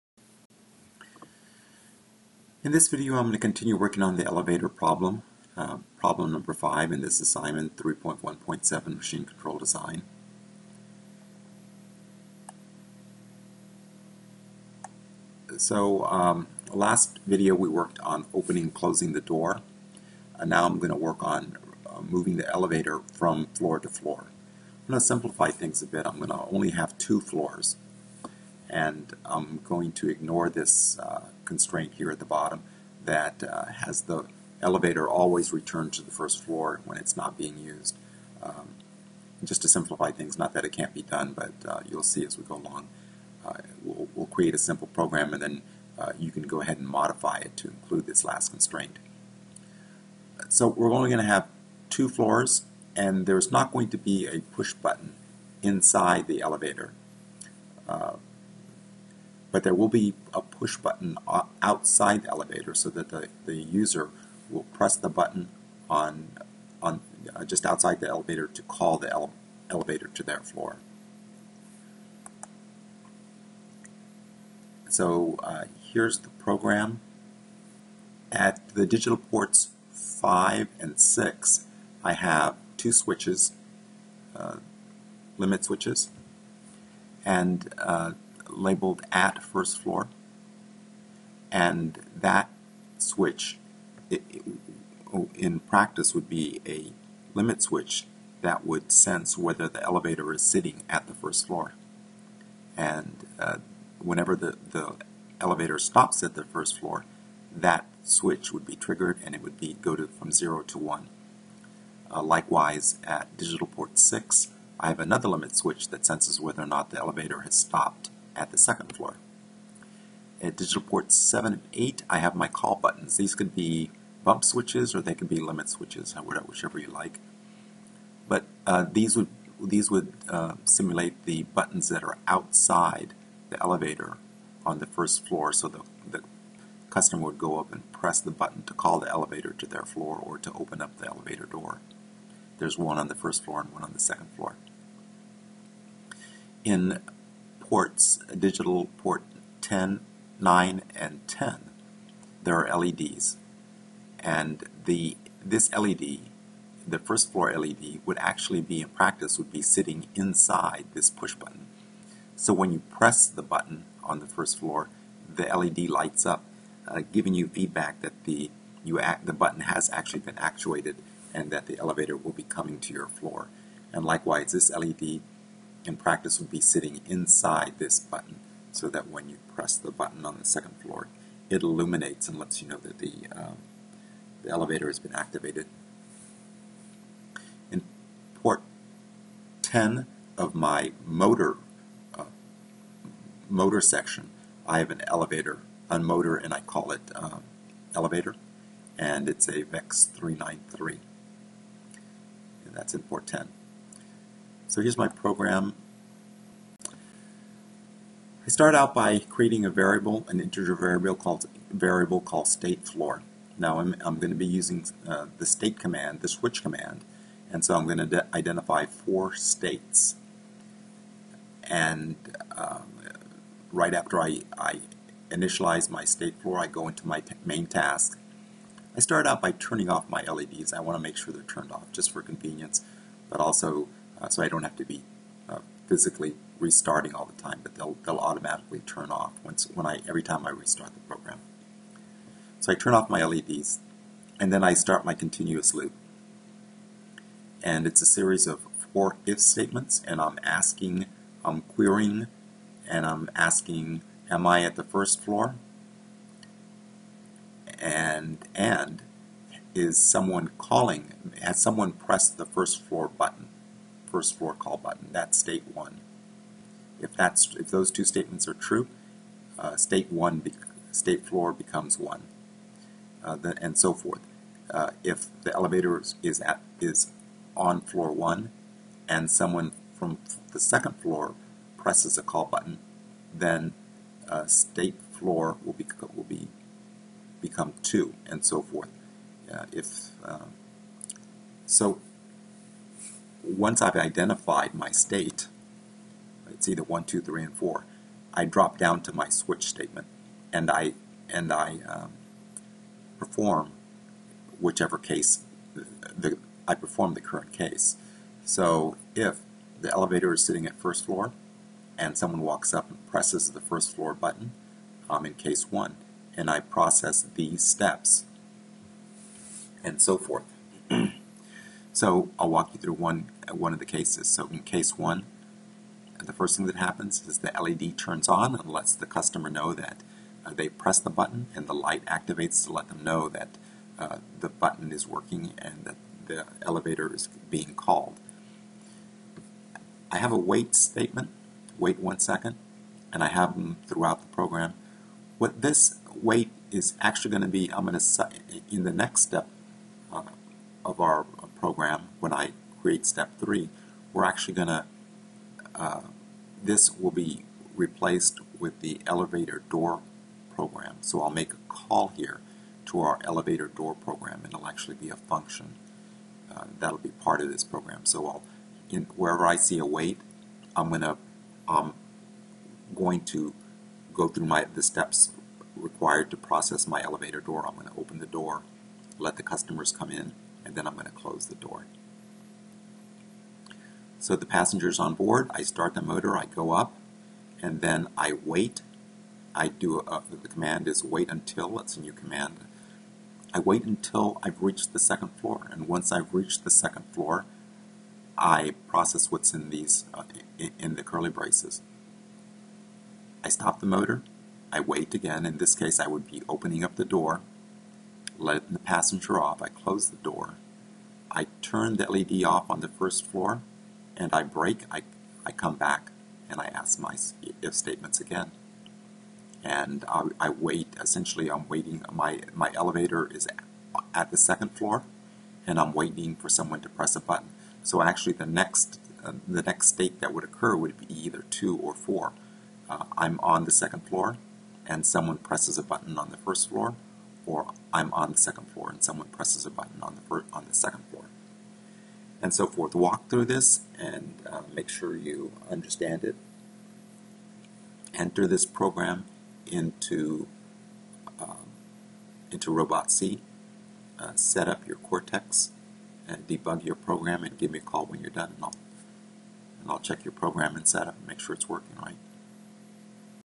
POE 3.1.7 Machine Control Design Problem 5 Part 2 Elevator Floor
PLTW POE 3.1.7 Machine Control Design Problem 5 Part 2 Elevator Floor